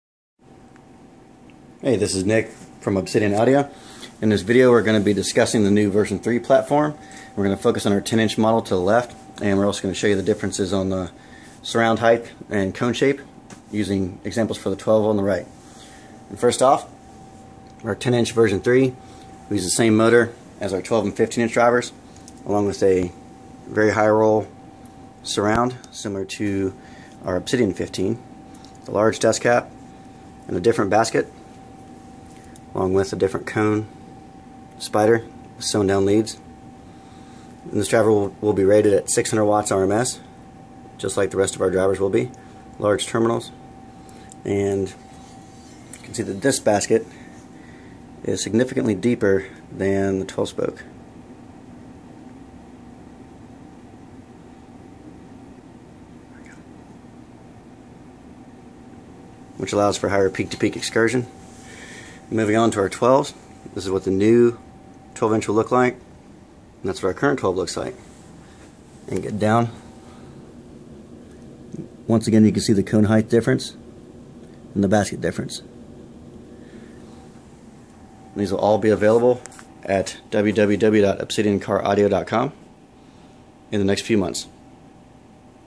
OA v3 video
Short descriptive video of the Obsidian v3 platform showing differences in baskets, cones, surrounds, etc.